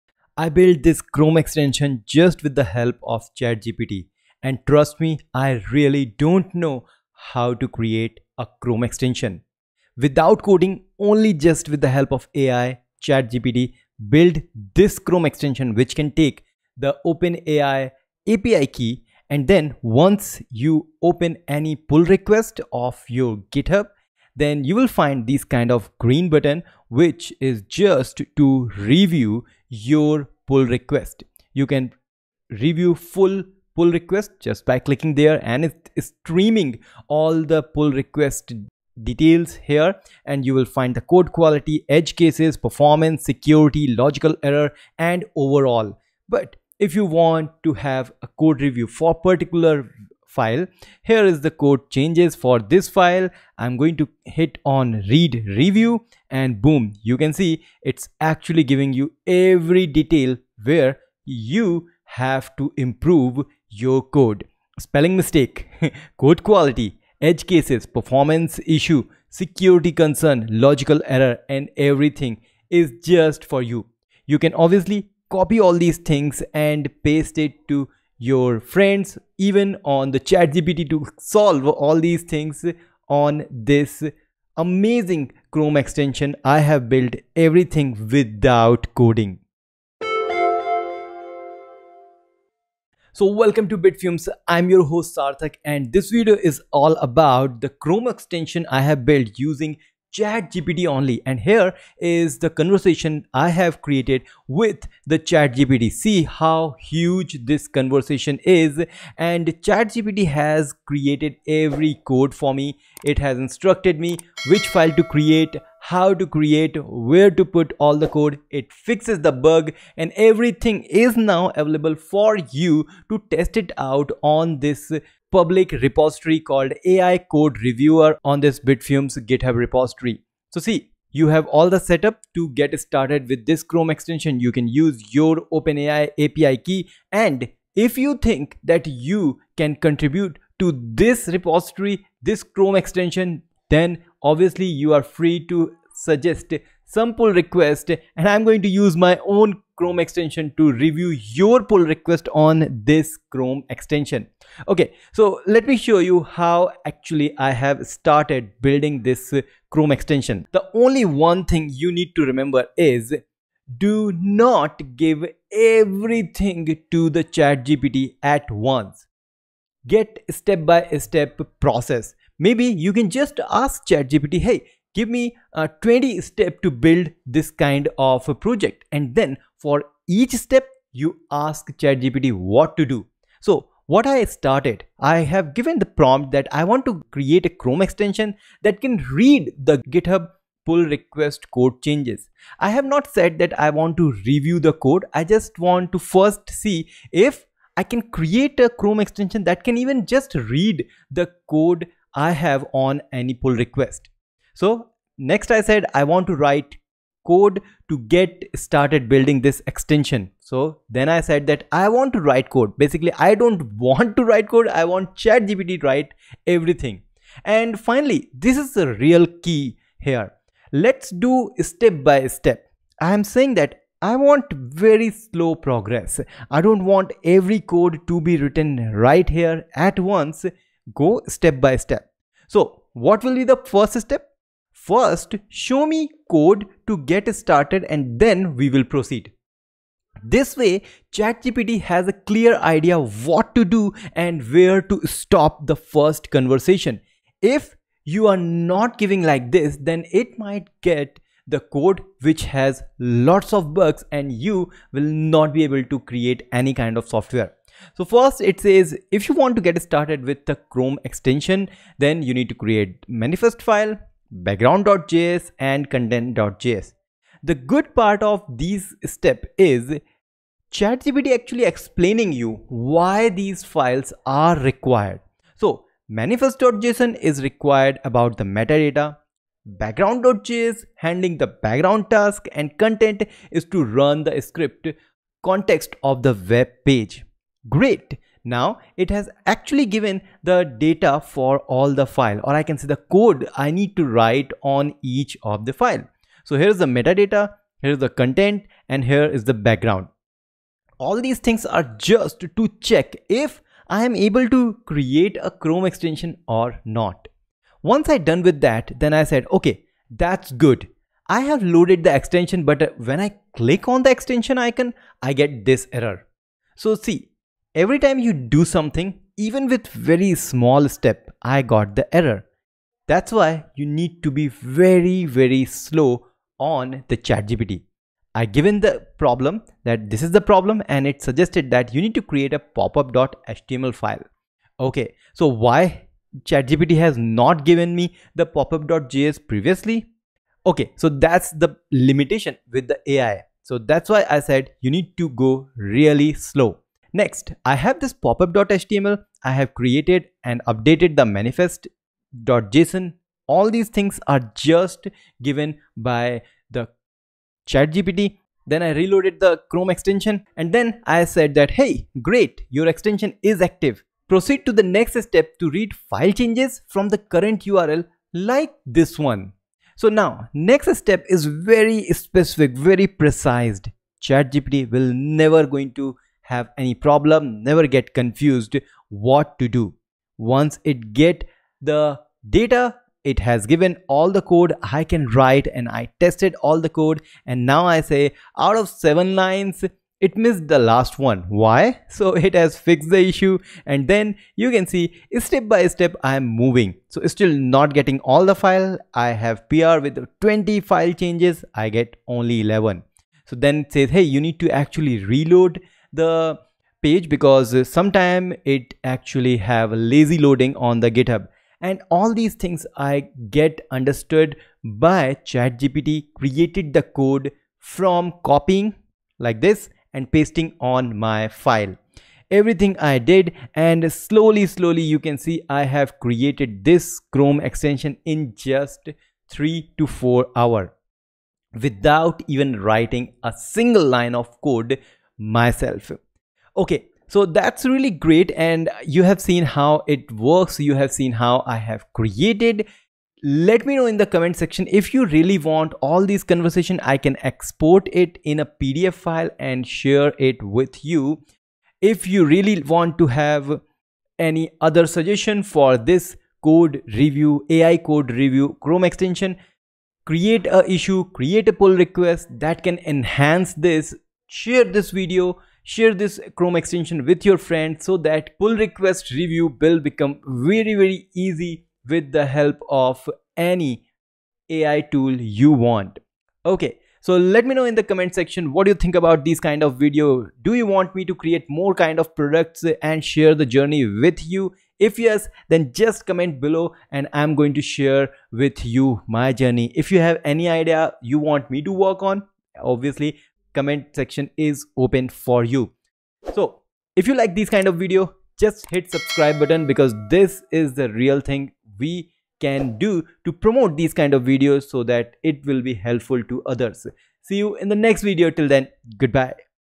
Build Chrome extension in 3 hours : No Coding only ChatGPT
In this video, I’ll show you how to build a Chrome extension in just 3 hours without writing a single line of code.
Whether you're a beginner or an experienced developer looking for a new approach, this tutorial will simplify the process and get your Chrome extension up and running in no time.

➡️Chapters:
0:00 Overview
2:34 Using ChatGPT to Generate the Extension Code
7:19 Conclusion and Next Steps

If you’ve found my content helpful and want to show your appreciation, consider buying me a coffee!